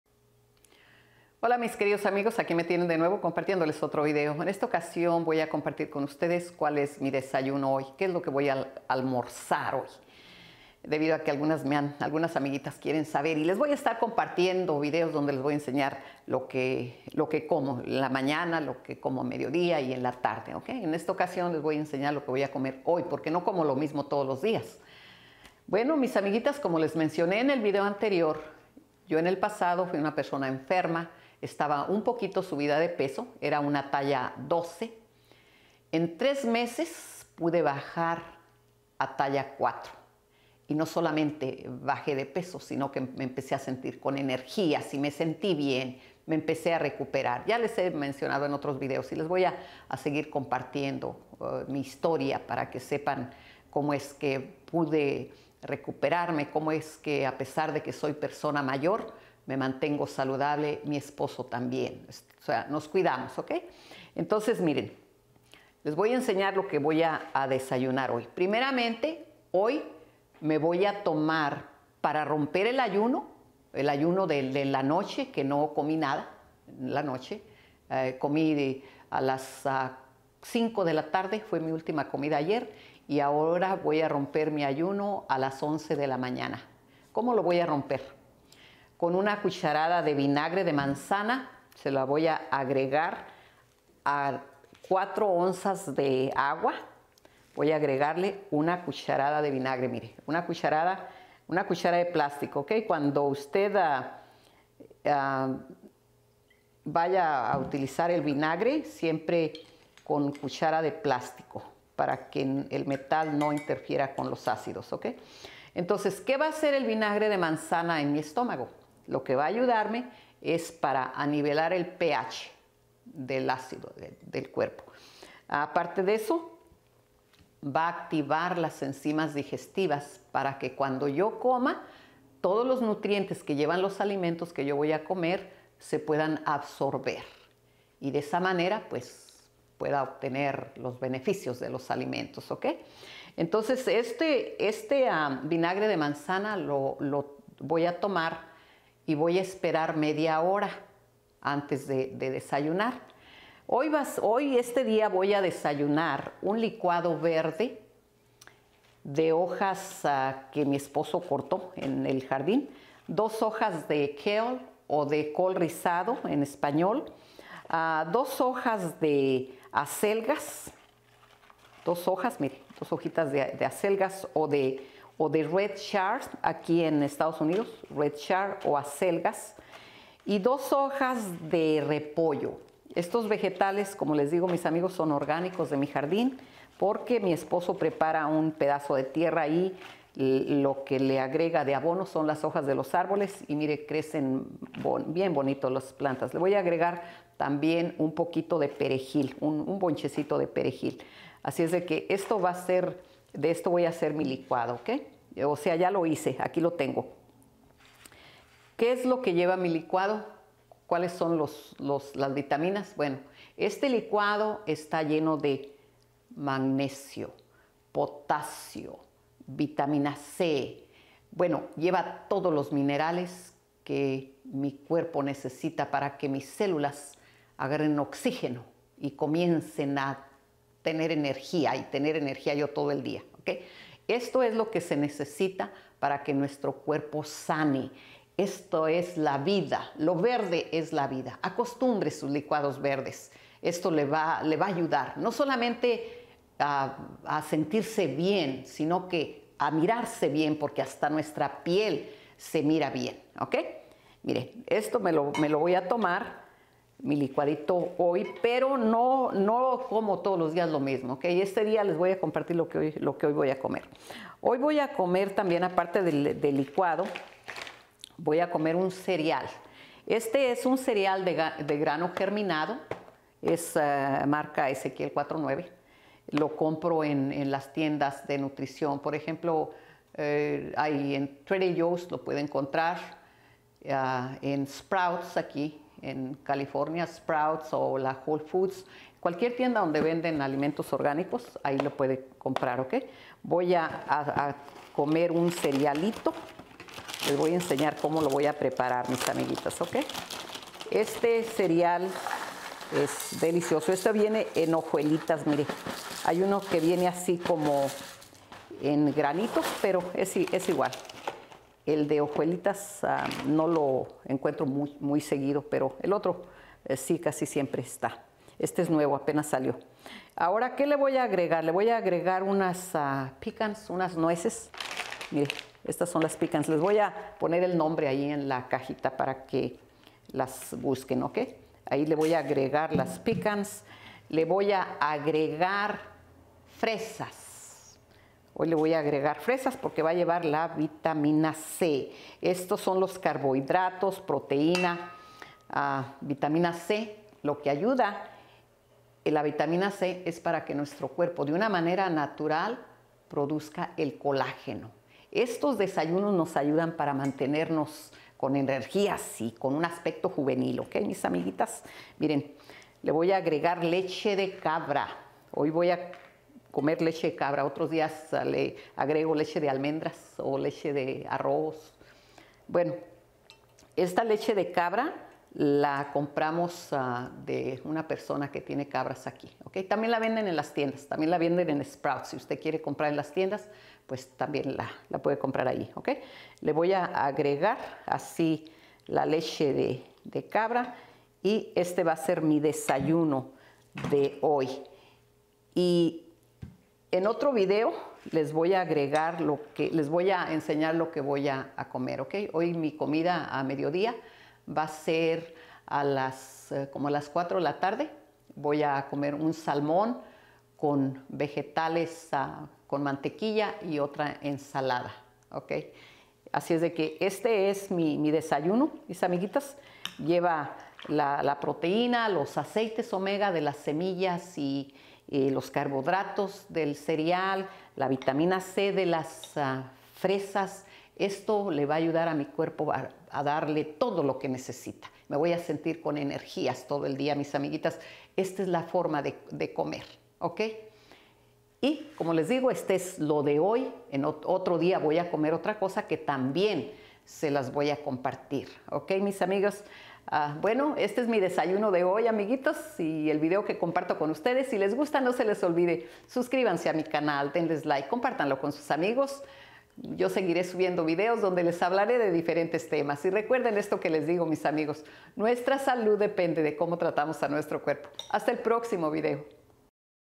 😍Éste desayuno me ayudó a bajar de talla 12 a talla 4 en 3 meses
Mi desayuno de hoy

rompí  mi ayuno con una cucharada de vinagre de manzana en cuatro onzas de agua

30 minutos después

Tome un licuado verde con
2 hojas de Kale ( col rosado)
2 hojas de red chard (acelgas)
2 hojas de repollo
1 Bonche de perejil

Cereal Ezequiel 4:9
Con fresas, nuez ( pecan)
Y leche de cabra